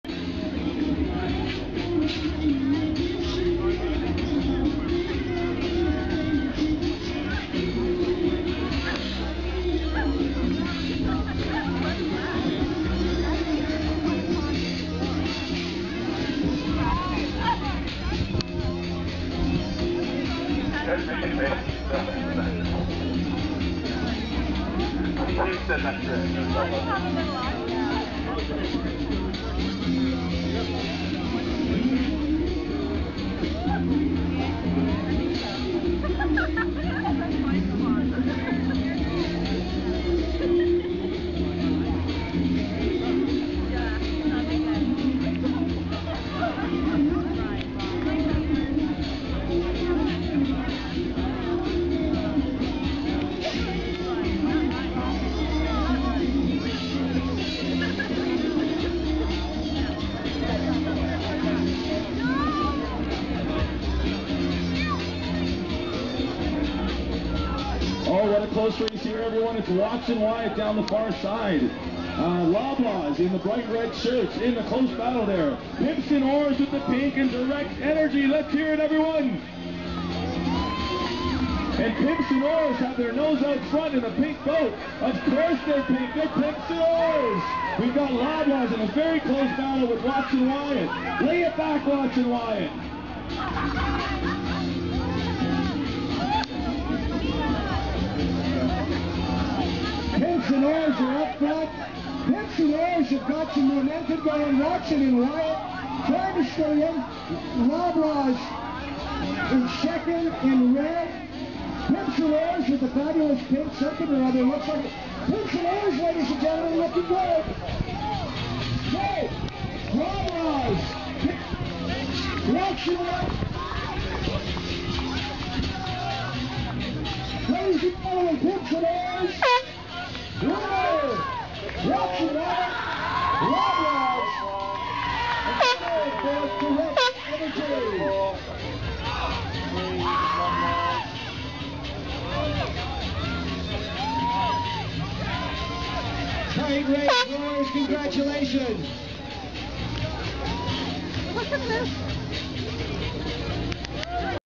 Toronto Viking Boat Race June 2007 - Semi-Finals
Toronto Viking Boat Race Petit Final from June 2007 - Team Direct Energy - 1st Race